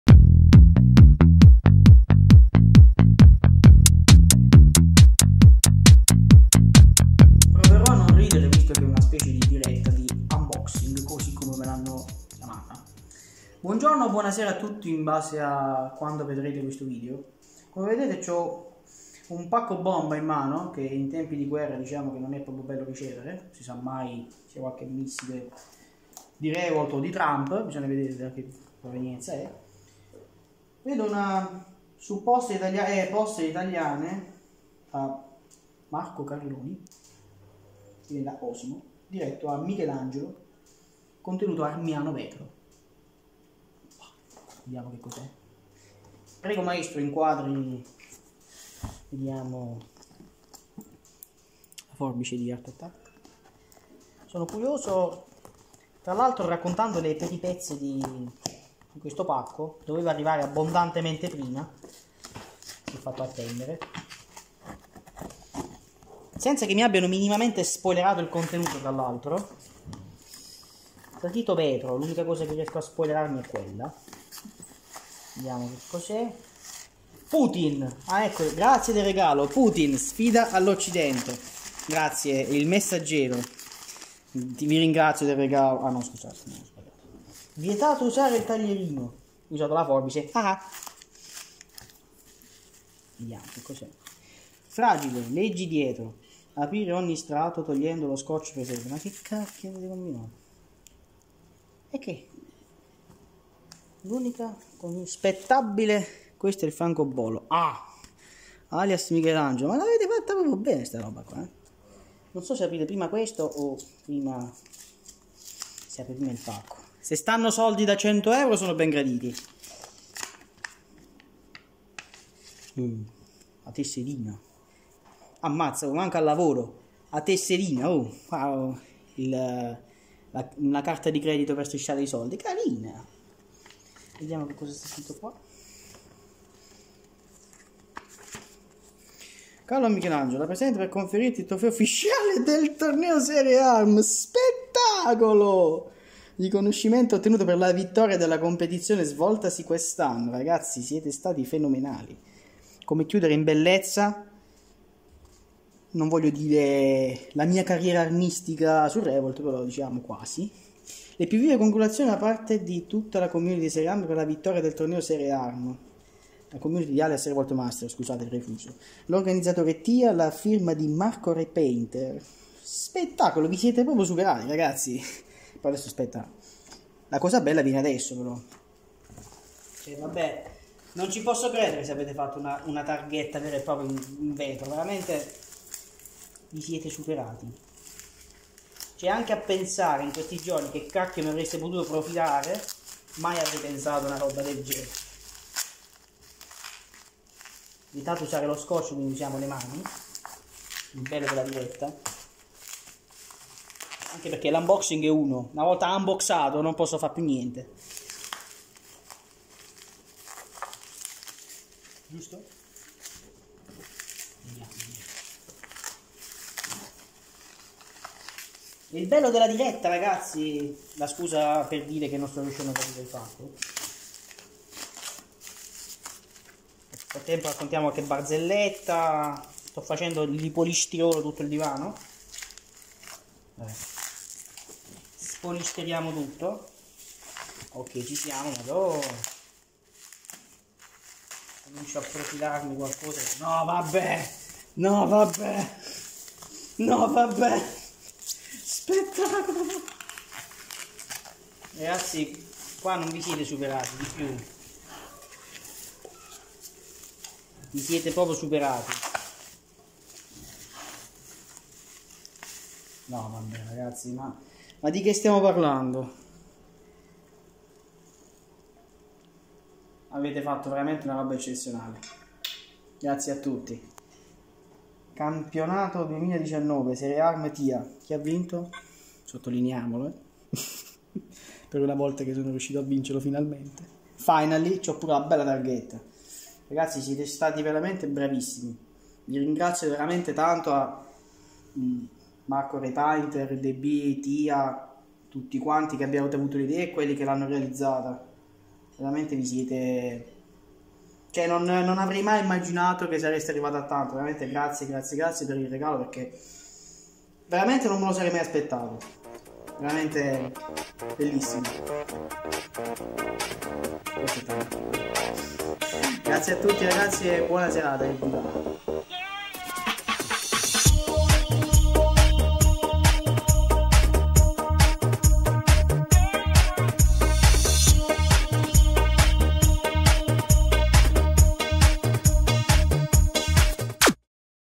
ARM unboxing - Serie ARM 2019
Primo unboxing di Alias Re-Volt Master, con premio per il primo classificato del campionato Serie ARM 2019.

Montaggio: Marco repainter